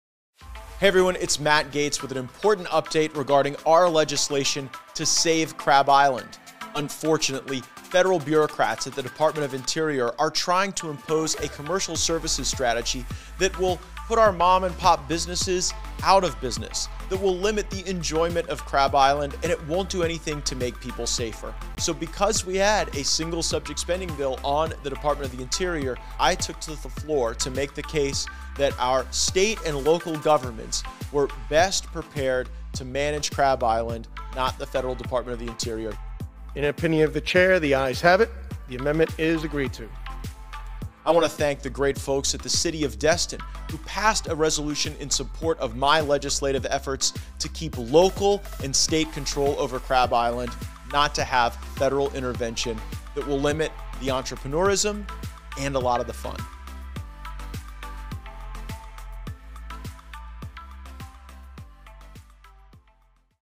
Crab Island Amendment PASSES in the House of Representatives!
I’m excited to share that my amendment to protect Crab Island from federal overreach in the Department of Interior appropriations bill has PASSED! ✅

Improving safety and responsible commerce on Crab Island is best managed in cooperation with local governments in Northwest Florida, not through ill-conceived federal overreach.

for ALL Firebrand content!